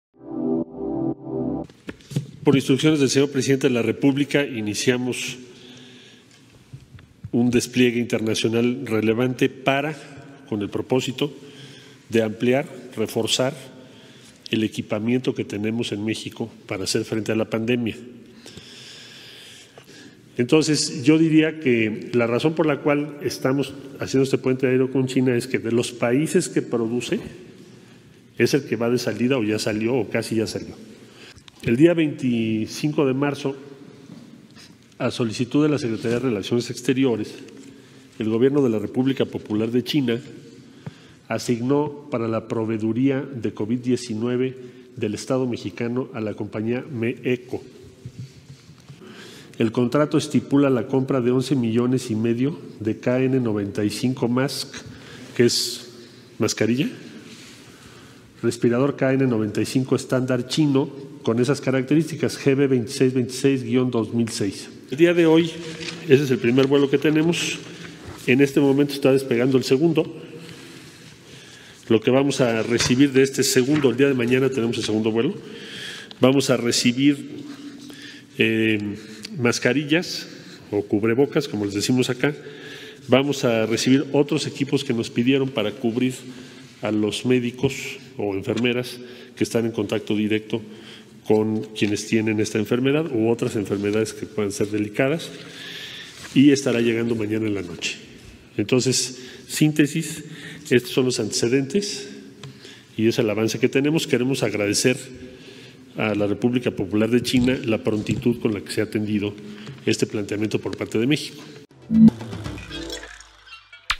México compra insumos médicos a china por más de 56 millones de dólares
Para abordar la crisis del COVID-19, el Gobierno mexicano compró insumos médicos a China por un valor de 56,4 millones de dólares, incluyendo 11,5 millones de máscaras respiradoras KN95, anunció este jueves el secretario de Relaciones Exteriores (SRE), Marcelo Ebrard.